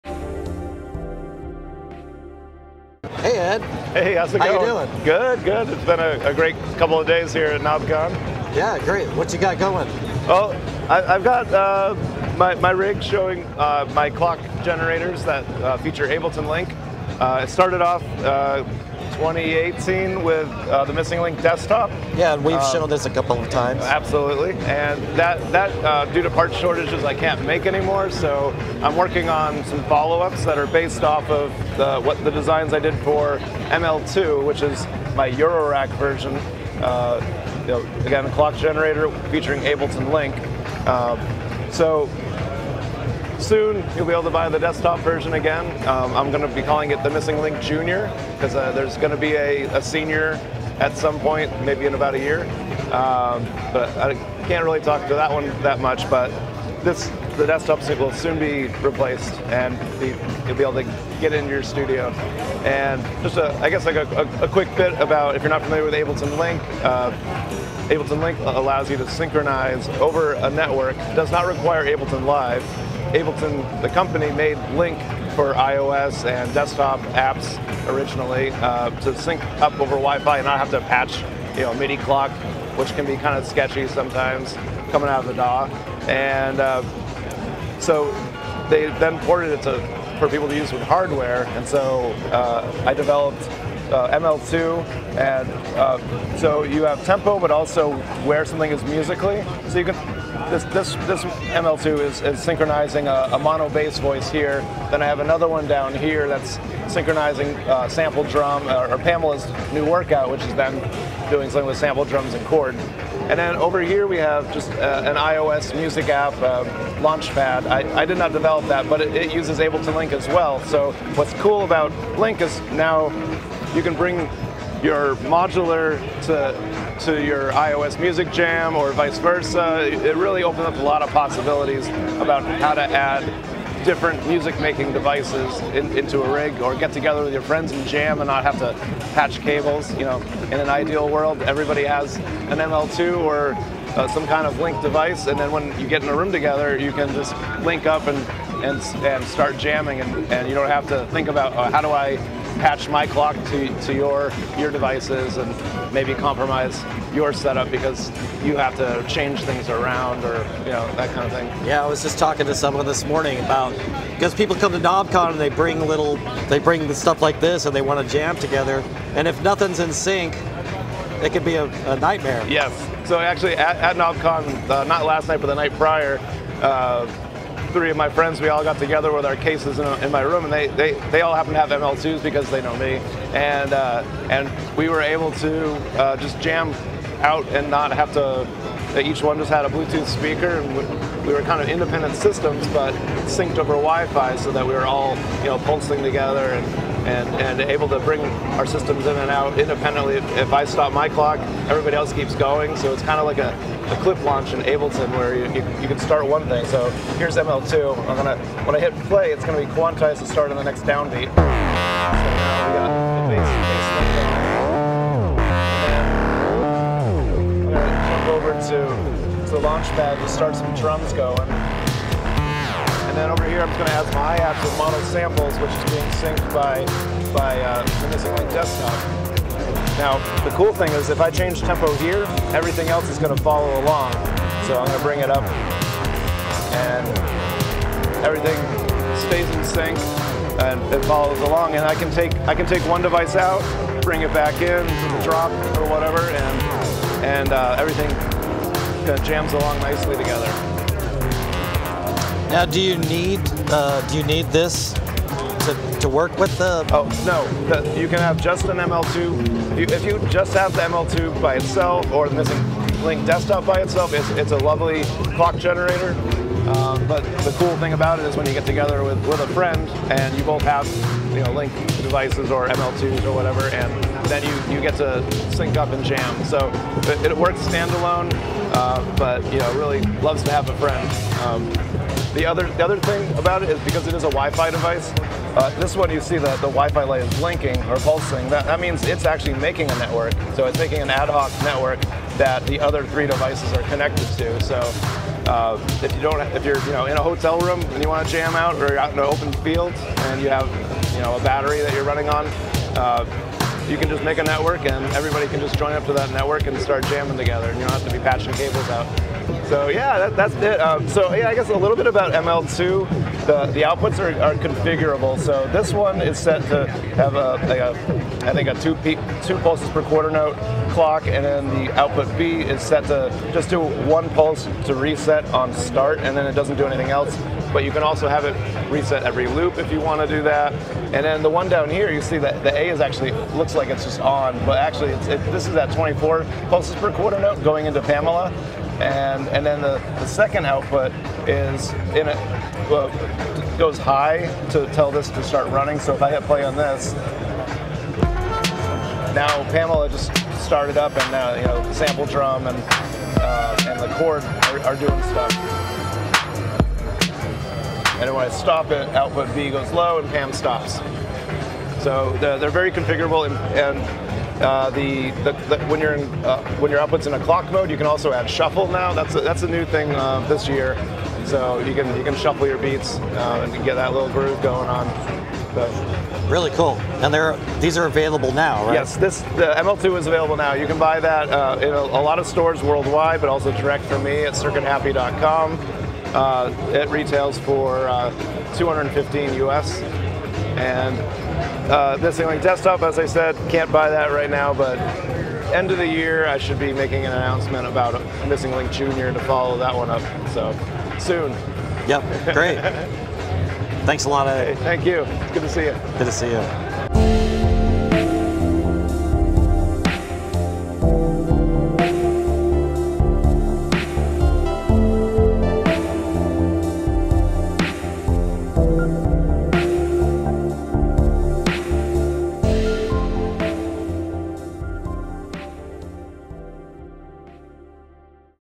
Knobcon 22: Circuit Happy Missing Link News
Its true that the part shortage has made the original Missing Link Ableton Live Link to Modular Clock device difficult to make but, Ed has been working on some new designs to ensure the next desktop device - the Missing Link Jr. will be coming soon - however the Eurorack ML-2 is available and can still interface with your  modular clock wirelessly from your Ableton session.